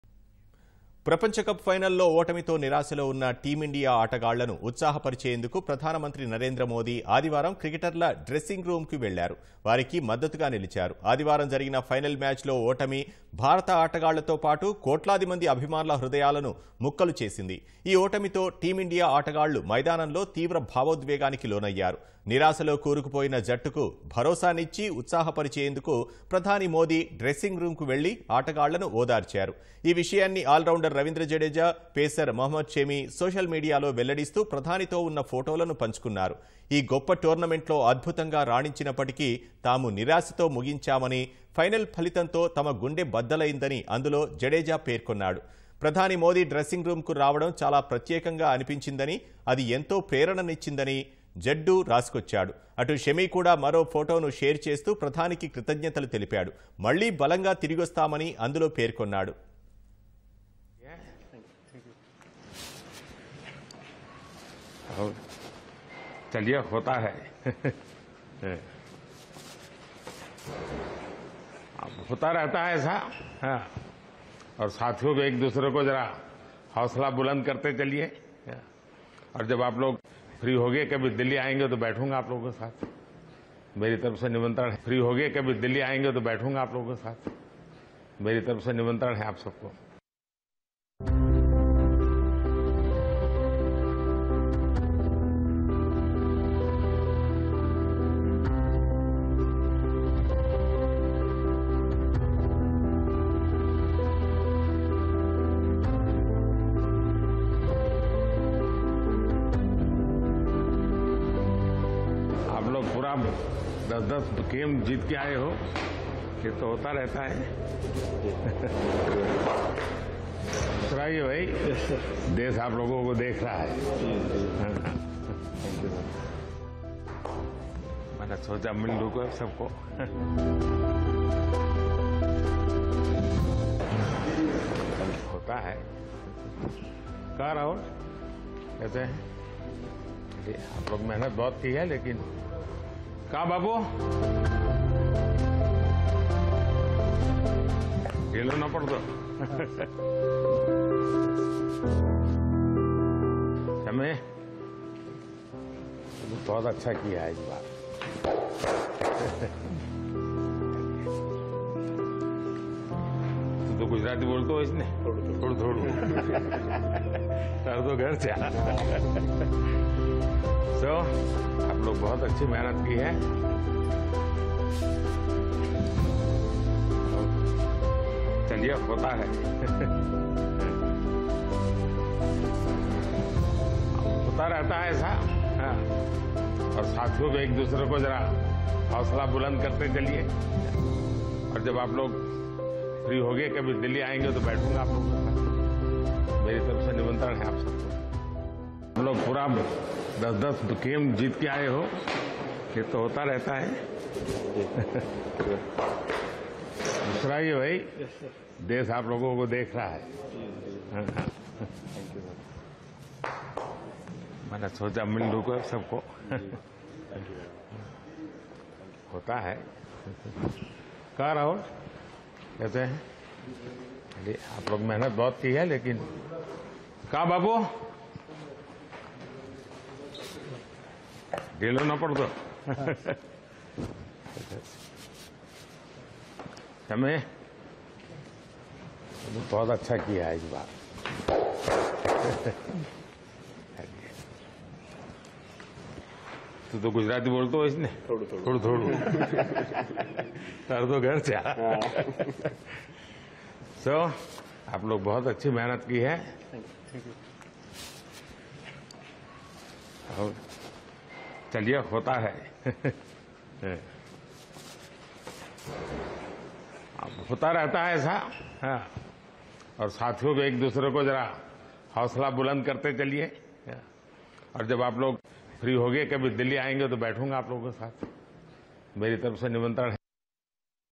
PM Modi Consoles Team India in Dressing Room after World Cup Final loss Against Australia
ప్రపంచకప్ ఫైనల్ లో ఓటమితో నిరాశలో ఉన్న టీమ్ ఇండియా ఆటగాళ్లను ఉత్సాహపరిచేందుకు ప్రధానమంత్రి నరేంద్రమోదీ ఆదివారం క్రికెటర్ల డ్రెస్సింగ్ రూమ్ కు వెళ్లారు. వారికి మద్దతుగా నిలిచారు. ఆదివారం జరిగిన ఫైనల్ మ్యాచ్ లో ఓటమి. భారత ఆటగాళ్లతో పాటు కోట్లాది మంది అభిమానుల హృదయాలను ముక్కలు చేసింది. ఈ ఓటమితో టీమ్ ఇండియా ఆటగాళ్లు మైదానంలో తీవ్ర భావోద్వేగానికి లోనయ్యారు. నిరాశలో కూరుకుపోయిన జట్టుకు భరోసానిచ్చి, ఉత్సాహపరిచేందుకు ప్రధాని మోదీ డ్రెస్సింగ్ రూమ్ కు వెళ్లి...ఆటగాళ్లను ఓదార్చారు. ఈ విషయాన్ని ఆల్ రౌండర్ రవీంద్ర జడేజా, పేసర్ మహమ్మద్ షమీ సోషల్ మీడియాలో వెల్లడిస్తూ ప్రధానితో ఉన్న ఫొటోలను పంచుకున్నారు. ఈ గొప్ప టోర్నమెంట్ లో అద్భుతంగా రాణించినప్పటికీ తాము నిరాశతో ముగించామని. ఫైనల్ ఫలితంతో తమ గుండె బద్దలైందని అందులో జడేజా పేర్కొన్నాడు. ప్రధాని మోదీ డ్రెస్సింగ్ రూమ్ కు రావడం చాలా ప్రత్యేకంగా అన్పించిందని. అది ఎంతో ప్రేరణనిచ్చిందని జడ్డూ రాసుకొచ్చాడు. అటు షమీ కూడా మరో ఫొటోను షేర్ చేస్తూ ప్రధానికి కృతజ్ఞతలు తెలిపాడు. మళ్లీ బలంగా తిరిగొస్తామని అందులో పేర్కొన్నాడు.